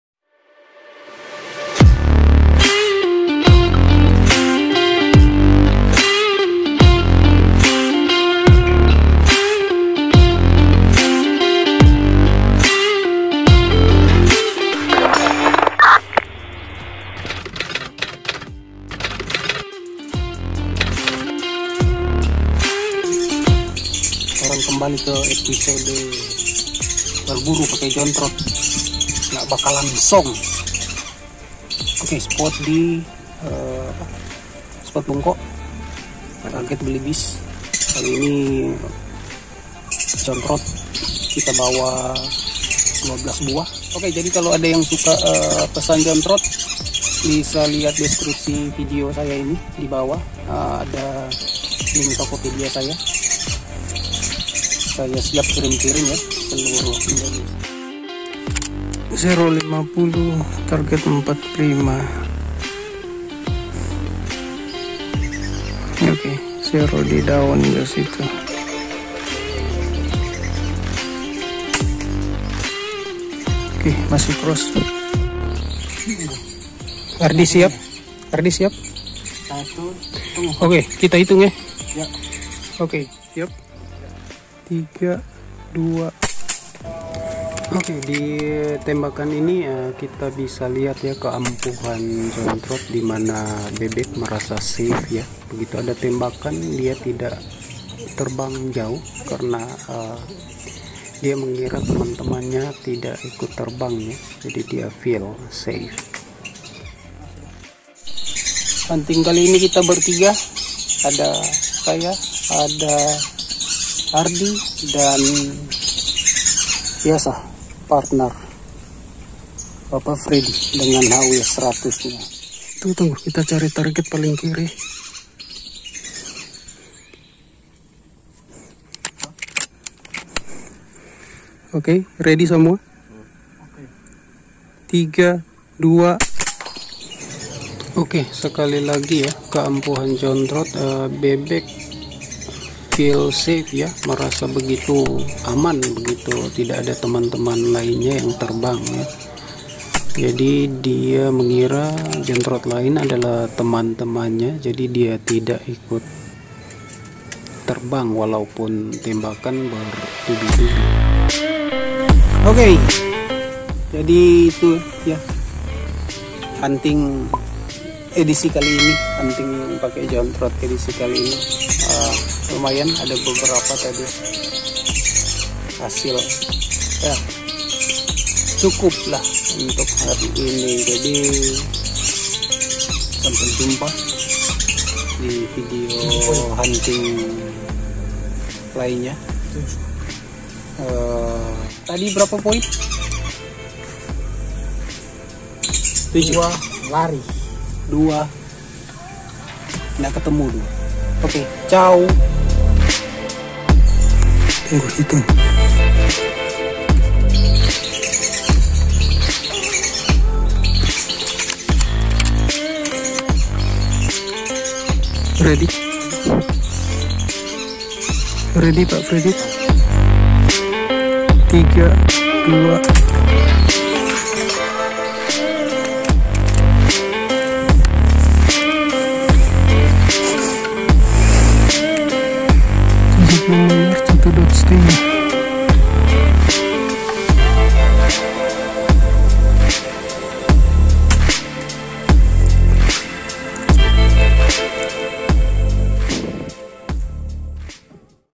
Hunting belibis pakai jontrot, gak bakalan ZONK!!! Eps.6 (Digempur Gak Kabur)
Ketemu lagi di eps .6, (hunting belibis pakai jontrot, gak bakalan ZONK!), kali ini saya hunting bertiga bersama rekan seclub saya, Fredy dan ardy,
7 point gusy, cukuplah!,
tonton keseruannya ya, 🙏👌.
My gear :
FX impact .177
Jsb heavy 10,34gr
Discovery HD 5-25x50 SFIR FFP DLTW
Xiaomi redmi note 9 pro
Selfmade mountcam
Pard 008
Eken H9R

Beli Jontrot Belibis ?
Bisa ke tokopedia saya dng cara
Click link dibawah 👇.

Beli Jontrot Belibis
di toko Artadina Rp150.000 di Tokopedia dengan Bebas Ongkir, Sekarang!